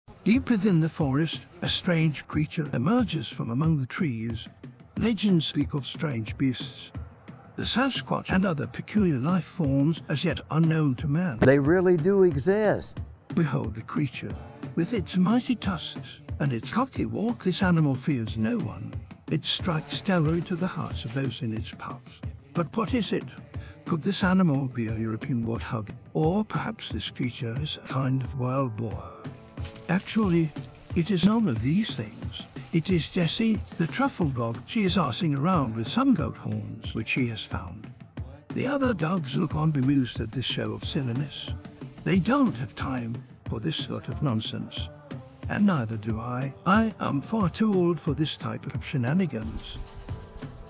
Strange Creature of the Truffle Forest - A fearsome tusked beast seen during a routine truffle hunt.
Get your free copy of our brand new truffle dog training guide.